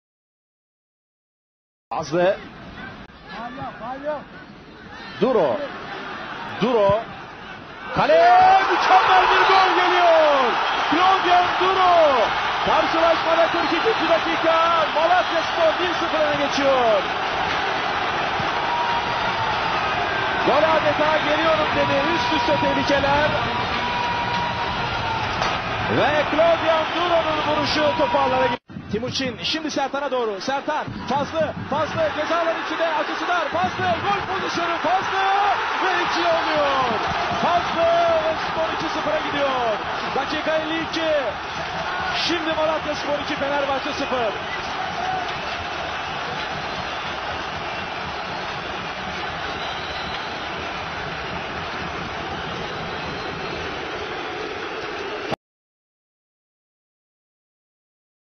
Malatyaspor'umuz 2 - 0 Fenerbahçe | Süper Lig
Süper Lig 15 Ekim 10/ 2002/2003 Sezonu
5  Hafta Maç Sonuçu Malatyaspor'umuz 2 - 0 Fenerbahçe

Kulübümüzün resmi sayfaları altaki linklere tıklayıp görebilirsiniz